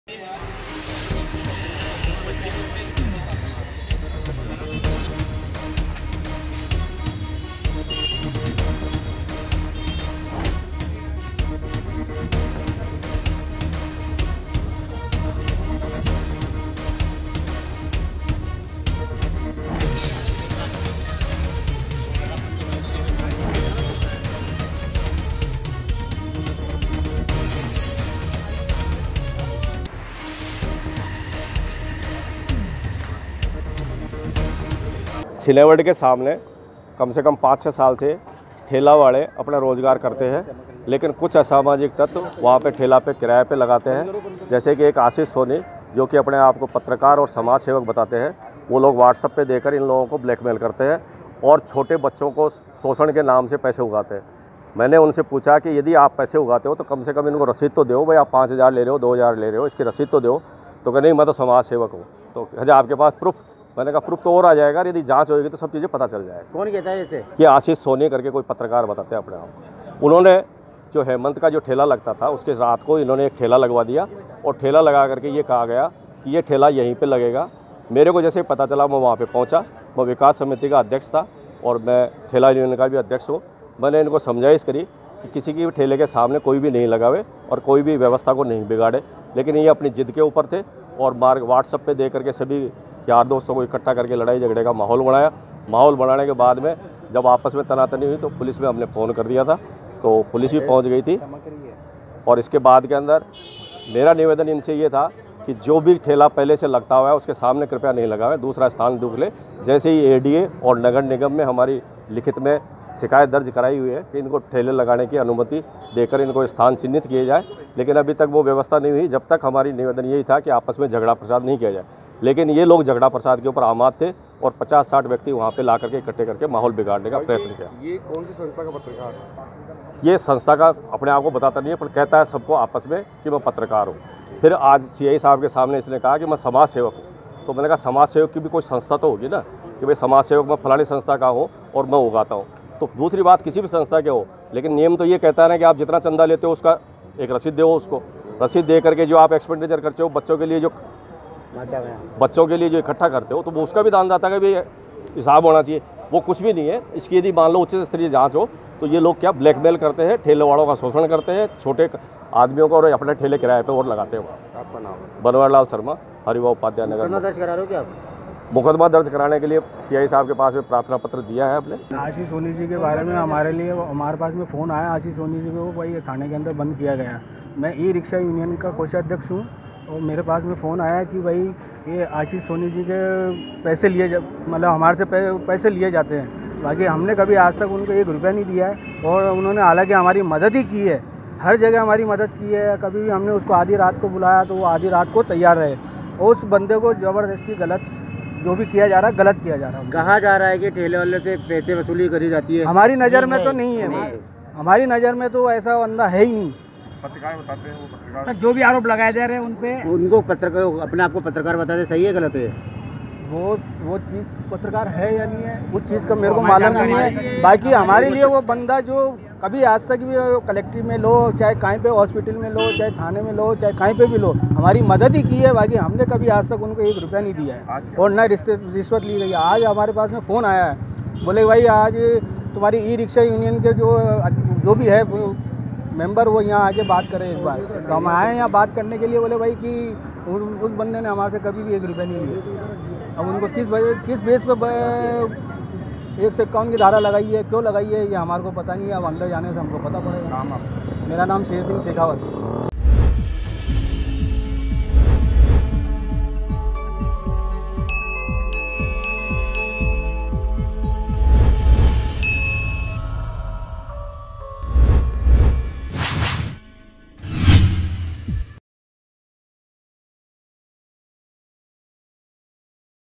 AJMER NEWS | समाजसेवी आशीष सोनी के खिलाफ व्यापारियों ने लगाए गंभीर आरोप | MTTV INDIA
समाजसेवी आशीष सोनी के खिलाफ व्यापारियों ने लगाए गंभीर आरोप
ठेला व्यापारियों का आरोप आशीष सोनी अवैध रूप से करता है वसूली
ठेला व्यापारियों का आरोप आशीष सोनी पत्रकार बनकर करता था वसूली
आशीष सोनी के समर्थन में आए ई रिक्शा चालक
गंज थाना पुलिस ने आशीष सोनी को शांति भंग के आरोप में किया गिरफ्तार